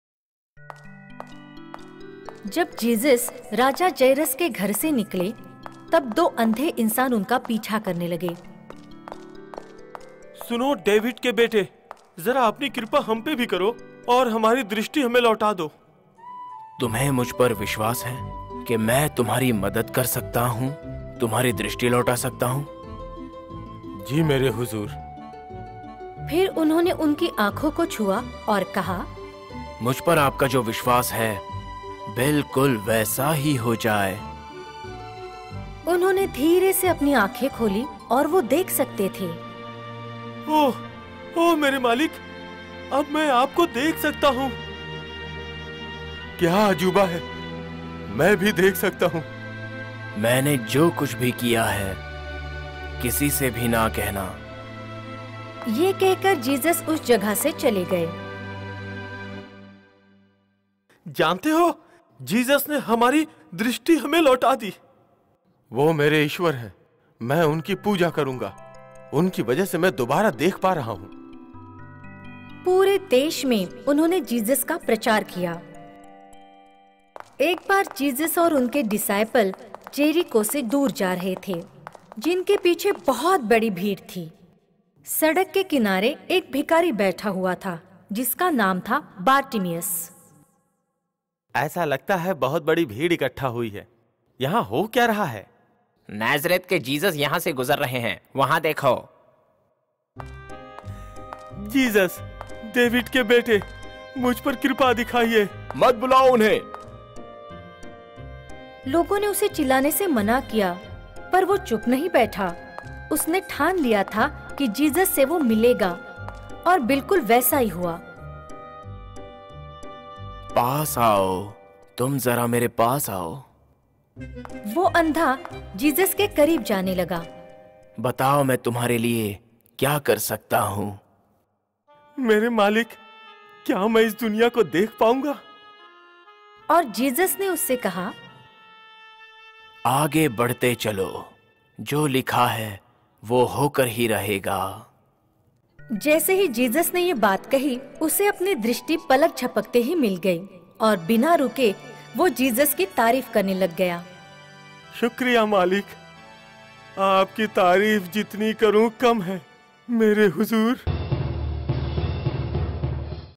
Bible Stories In Hindi Vol. 2 - अंधे पुरुषों को ठीक करना | Healing Blind Men | God Stories For Kids
"God Stories For Kids - New Bible Stories Vol 2 - Healing Blind Men - अंधे पुरुषों को ठीक करना - बच्चों के लिए एनिमेटेड कार्टून कहानियां।

एक बार जीसस जेरिको  में चल रहा था जब दो अंधे आदमी उसके पीछे चलना शुरू कर दिया | जीसस ने उन्हें ठीक किया और वे फिर से देख सकते थे | बाद में उन्होंने हर जगह जीसस के बारे में बात की और कई लोग अब जानते थे। एक अंधा भिखारी बैठा था और जीसस की प्रतीक्षा कर रहा था | वह उसके प्रति चलना शुरू कर दिया। जीसस ने उससे उस पर विश्वास रखने के लिए कहा और फिर उसकी आंखों को ठीक किया। यह जीसस का एक और चमत्कार था |

Once Jesus was walking in Jericho when two blind men started following him. Jesus healed them and they could see again. Later they spoke of Jesus everywhere and many people knew now. A blind beggar was sitting and was waiting for Jesus. He started walking towards Jesus. Jesus asked him to keep faith in him and then healed his eyes. This was another miracle of Jesus.

TIME STAMP: 1:59 - Jesus heals a blind beggar who has faith in him.